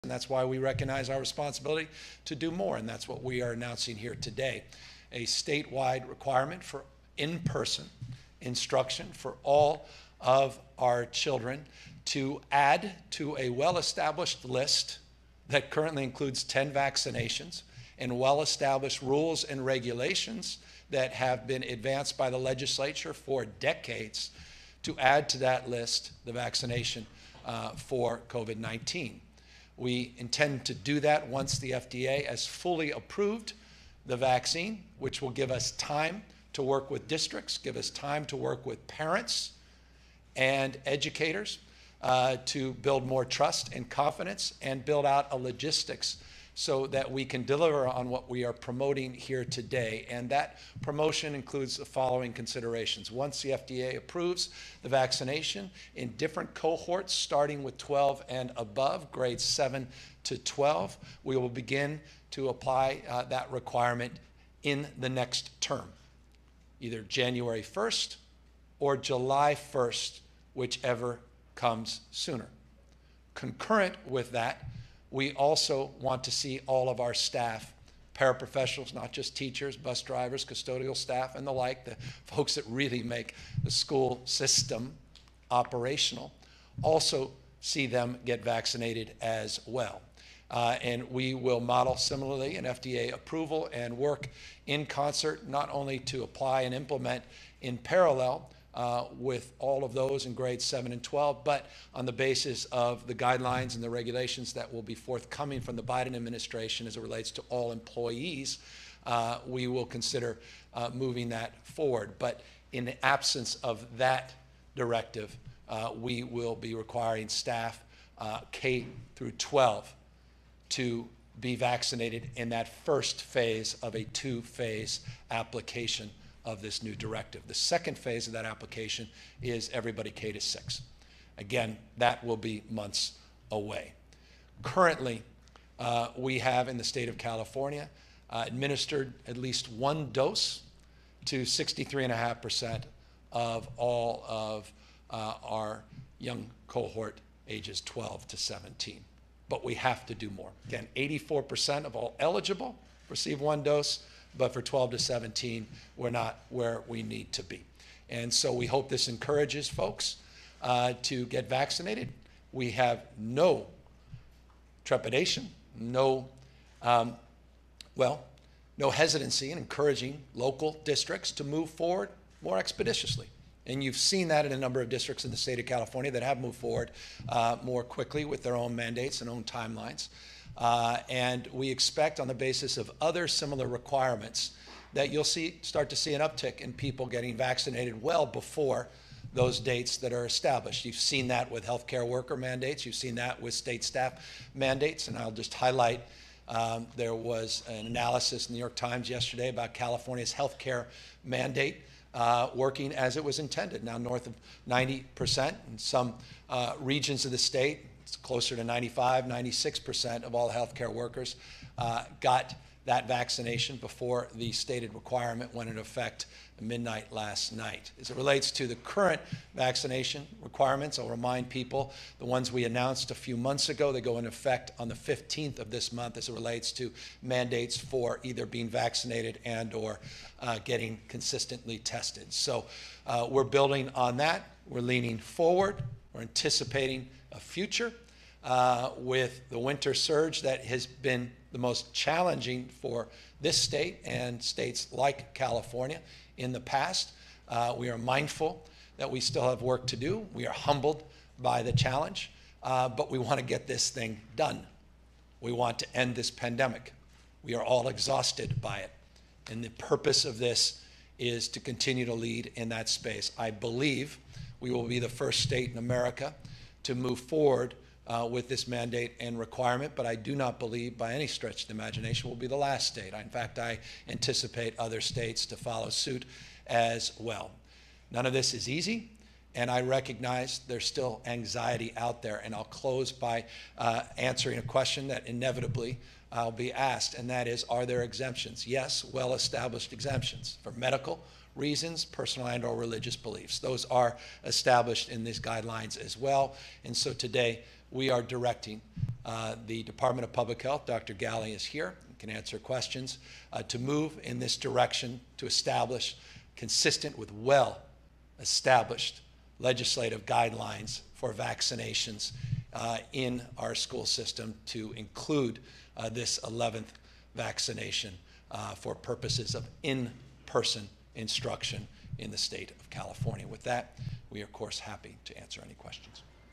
California Will Be First State Requiring Covid-19 Vaccines For All K-12 Students
California Gov. Gavin Newsom announced Friday the state will require all students in K-12 schools to get vaccinated against Covid-19 once the vaccines are given full approval for use in children under the age of 16 by the Food and Drug Administration (FDA), making it the first state to do so.